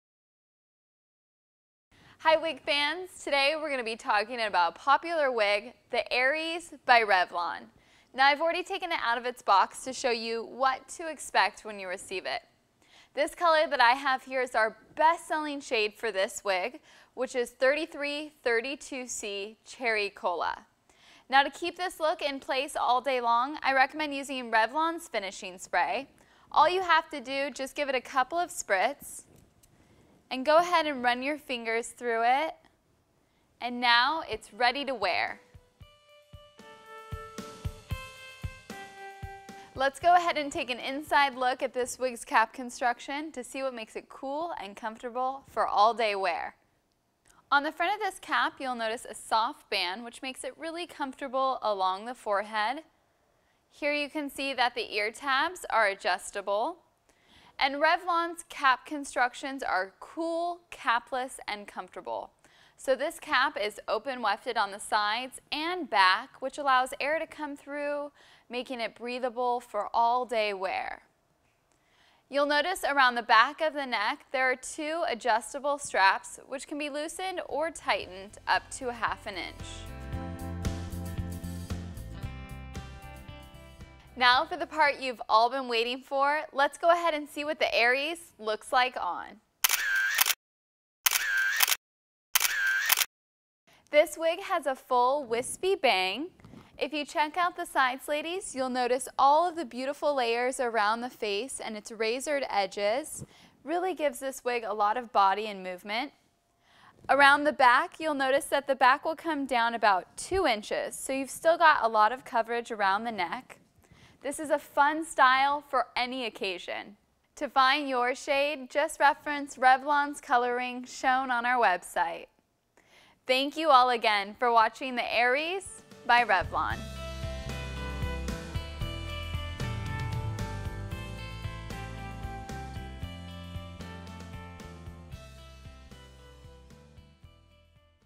Revlon Aries Wig Review
Aries is a layered short wig with razored edges. This modern style synthetic hair piece by Revlon has flipped out hair along the nape. Featuring Revlon C3 (Cool, Comfortable, Capless) technology this wig is so soft and comfortable, you won't even know it's there.